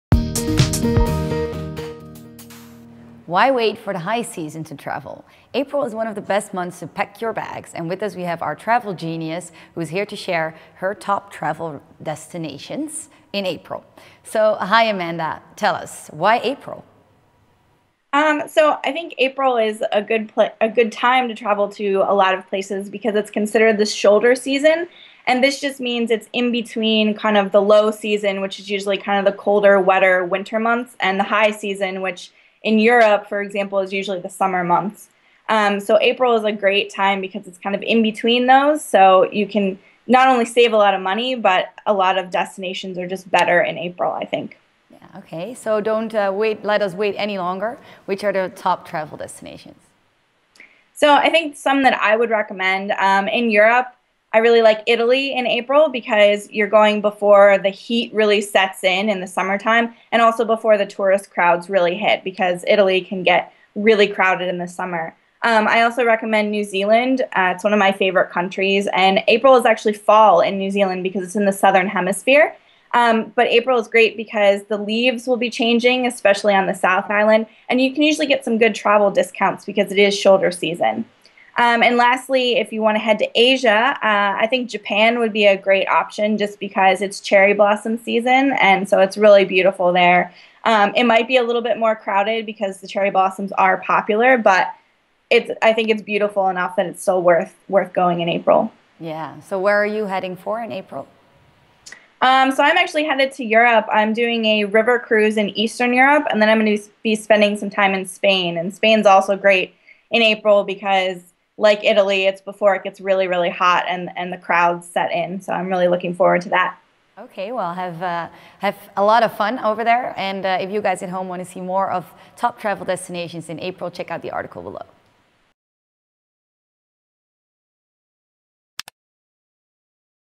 April is Awesome For Travelling, This Is Why And Where To Go
Forget those hot, mosquito invested, crowded summer months. April is the new August, and today with us in the studio is Amanda, the solo female traveller.

We talked to Amanda before about travelling alone as a lady. She gave us tips on how to stay safe. Today she provides us with some destinations that are awesome to visit in April. So drop your laptop/books/fifth cup of coffee and run to the travel agency: you are travelling!

April is great because:
Hotels, restaurants, shops and restaurants are not enjoying the tourist masses and may still have normal priced.
For many countries, for example the ones in Europe, the heat has not set in yet.  In Italy and Spain for instance, the temperatures may rise to boiling hot (especially in the cities) in summer.
Where it is spring in Europe, it’s fall in the Southern Hemisphere. New Zealand makes for an awesome travel destination according to Amanda. Changing leaves create for amazing scenery. This also makes Japan an ideal travel destination: April is cherry blossom season.

So pack your bags, what are you waiting for? April is waiting for you!